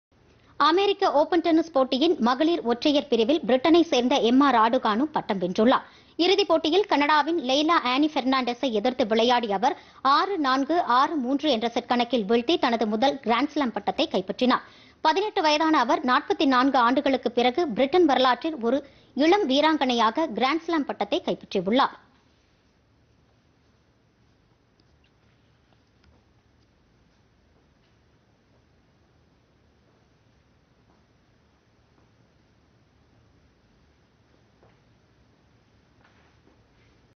யு எஸ் ஓபன் - மகளிர் ஒற்றையர் சாம்பியன்PodhigaiTamilNews
அமெரிக்க ஓபன் டென்னிஸ் போட்டி -மகளிர் ஒற்றையர் சாம்பியன் பட்டம் வென்றார் எம்மா ராடுகானு